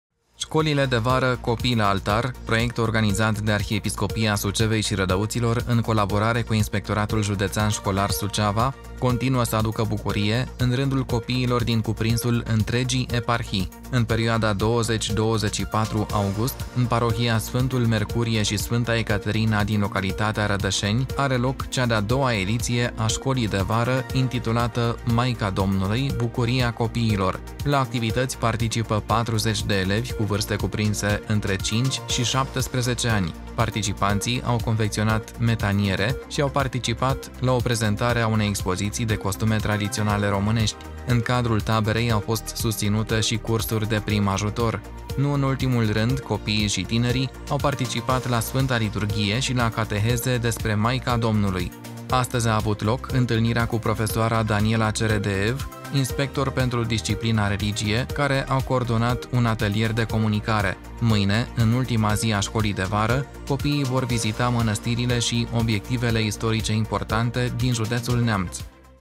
Școala de vară „Maica Domnului, bucuria copiilor”, la Rădășeni
Școlile de vară „Copii la Altar”, proiect organizat de Arhiepiscopia Sucevei și Rădăuților, în colaborare cu Inspectoratul Județean Școlar Suceava, continuă să aducă bucurie în rândul copiilor din cuprinsul  întregii eparhii. În perioada 20 – 24 august, în Parohia „Sfântul Mercurie și Sfânta Ecaterina” din localitatea Rădășeni, are loc cea de-a doua ediție a Școlii de vară intitulată „Maica Domnului, Bucuria copiilor”. La activități participă 40 de elevi cu vârste cuprinse între 5 și 17 ani. Participanții au confecționat metaniere și au participat la o prezentare a unei expoziții de costume tradiționale românești. În cadrul taberei au fost susținute și cursuri de prim ajutor. Nu în ultimul rând, copiii și tinerii au participat la Sfânta Liturghie și la cateheze despre Maica Domnului. Astăzi a avut loc întâlnirea cu profesoara Daniela Ceredeev, inspector pentru disciplina Religie, în cadrul Inspectoratului Județean Suceava, care a coordonat un atelier de comunicare. Mâine, în ultima zi a școlii de vară, copiii vor vizita mănăstirile și obiectivele istorice importante din județul Neamț.